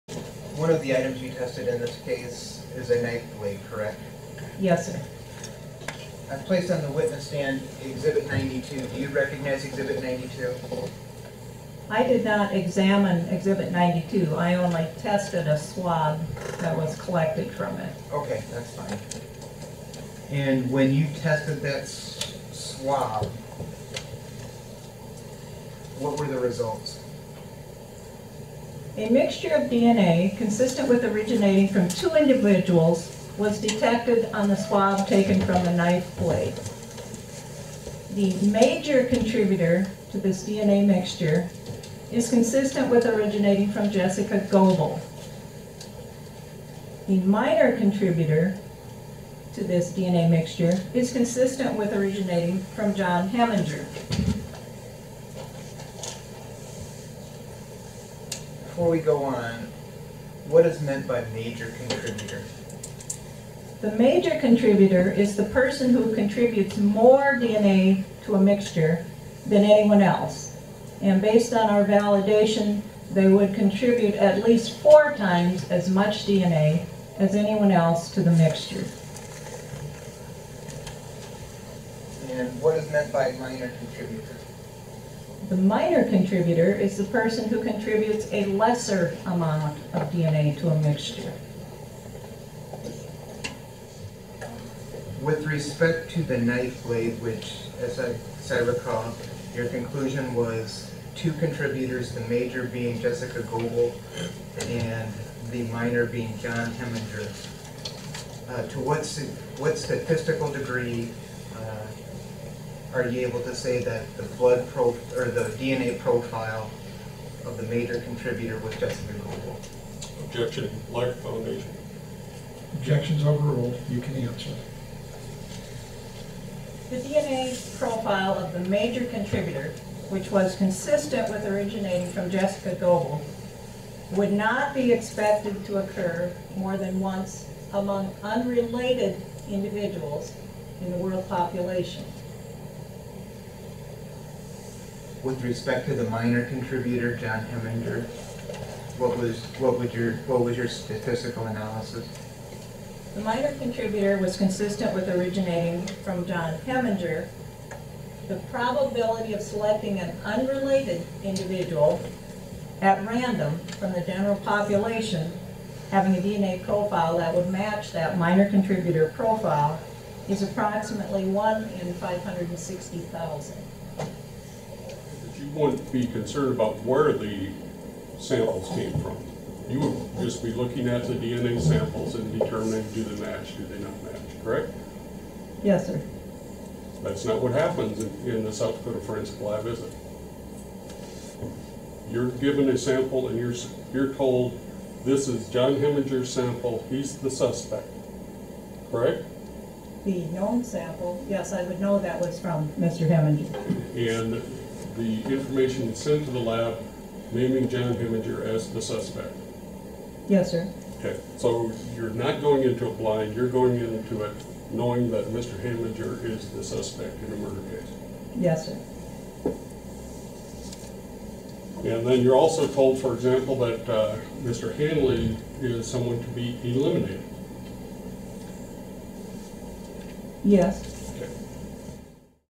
Key testimony: Forensic scientist Kandy Smith talks DNA evidence
Kandy Smith with the South Dakota Forensic Lab talks about DNA evidence in the John E. Hemminger case. Hear full testimony in our replay of Monday’s afternoon session at AberdeenNews.com.